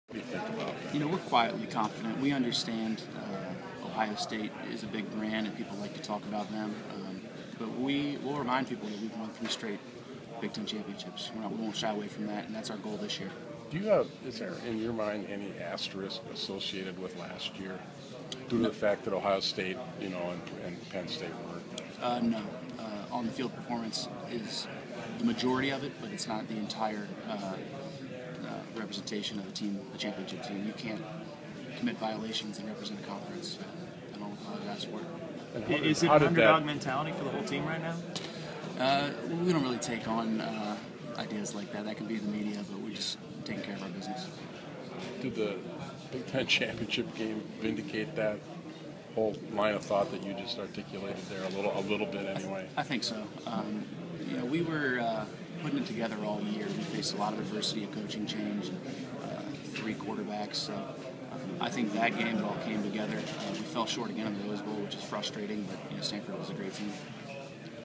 Badgers LB Chris Borland
Badgers LB Chris Borland talks about being overlooked in favor of Ohio State when it comes to Big Ten Conference favorite.